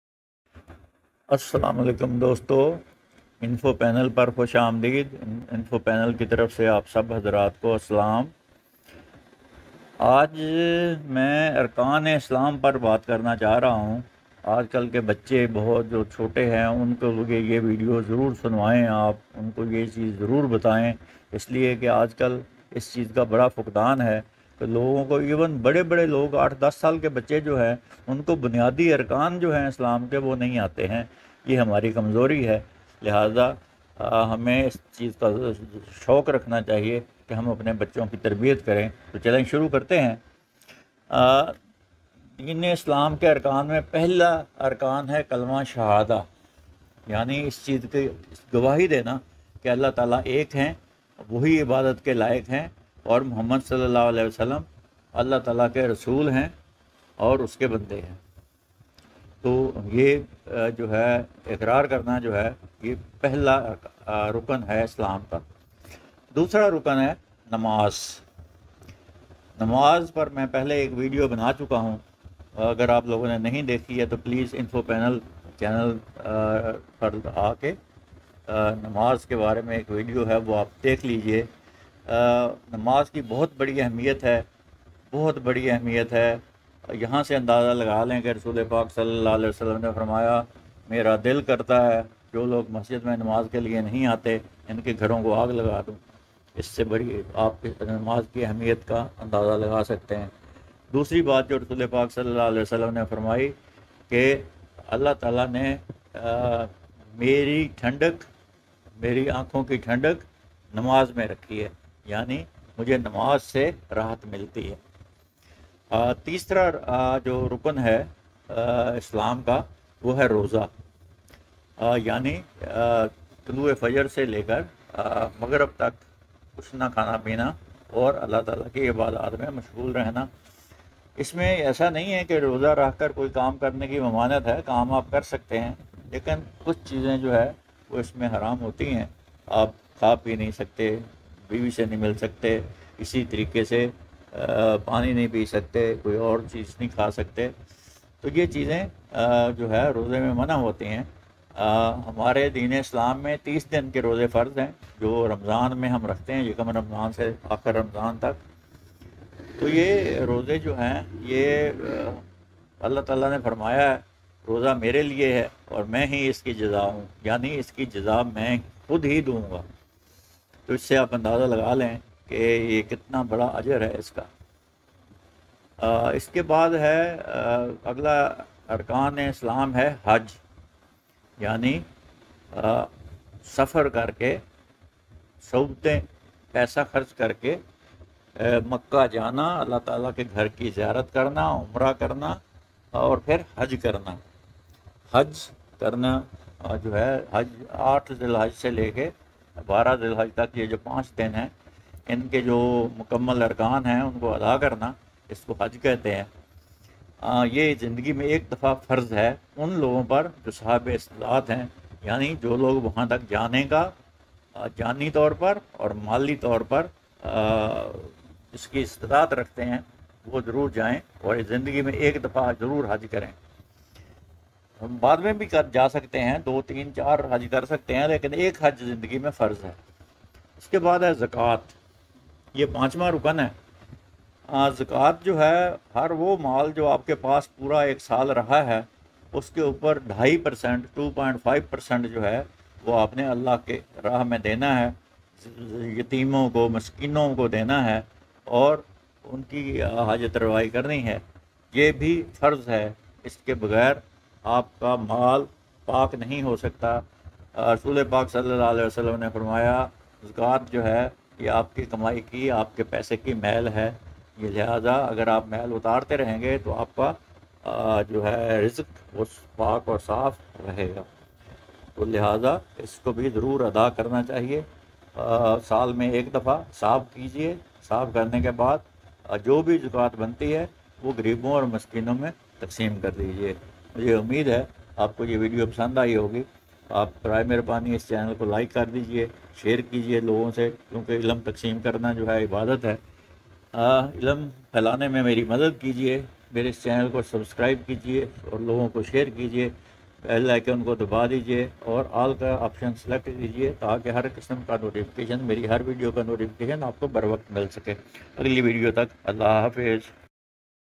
Islam kay bunyadi arkaan | Info panel
Basic pillars of Islam are described in this video. pleade watch video and refresh your eimaan.
Islam_kay_bunyadi_arkaan Pillars_ of_Islam.
Info Panel